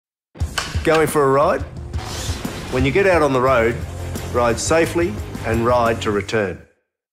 Mick Doohan - Ride to Return - Rider Safe
Haven't ridden a motorcycle for a while? Make sure you're prepared to safely get back out on the road. Do a Returning Rider Course and Ride to Return.